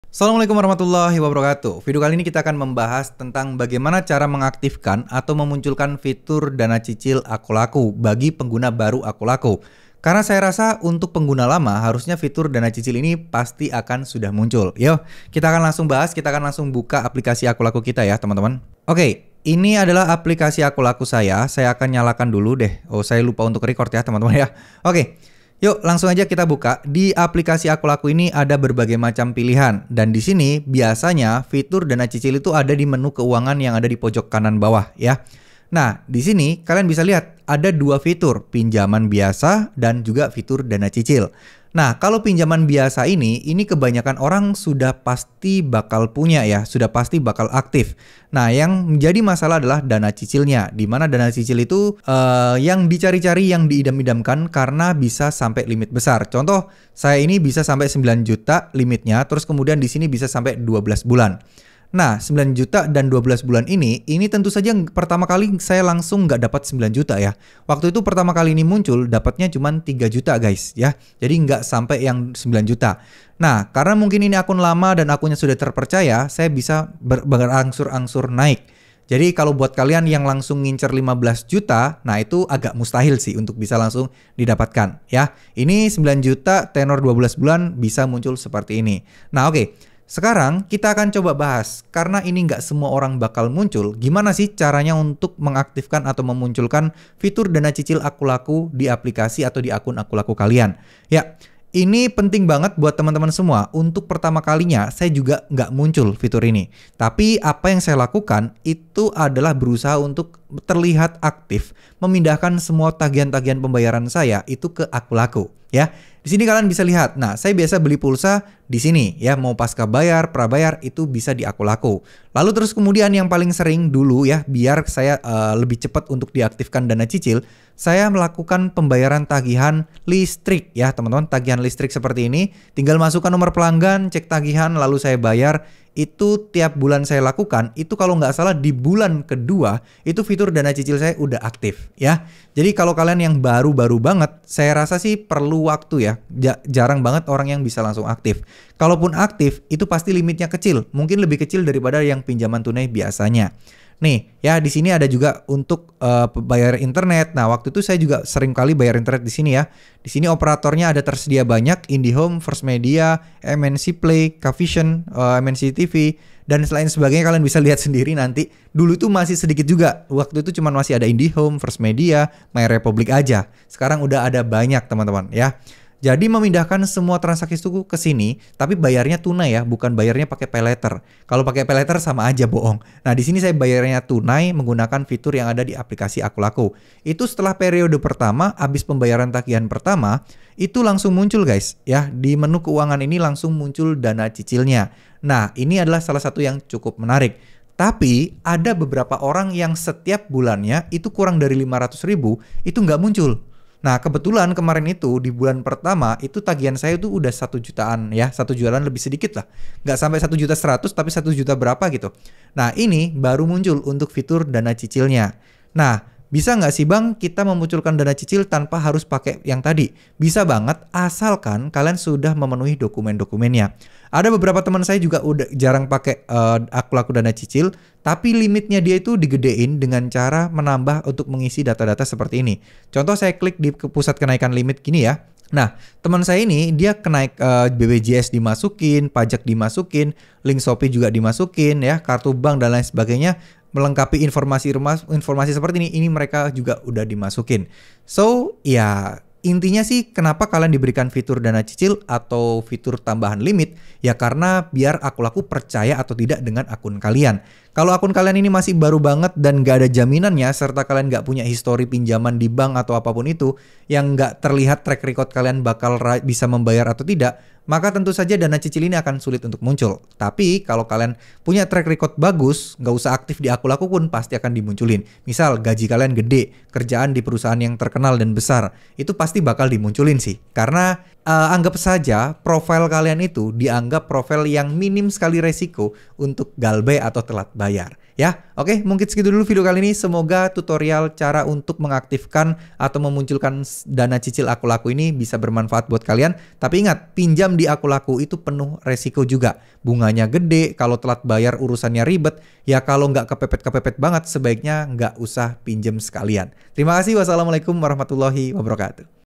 Cara Memunculkan Dana Cicil Akulaku Terbaru, Dana Cicil Akulaku Tidak Muncul!
Cara Memunculkan Dana Cicil Akulaku Terbaru, Dana Cicil Akulaku Tidak Muncul! Bagaimana cara aktifkan dana cicil akulaku? dan bagaimana cara untuk buka fitur dana cicil akulaku.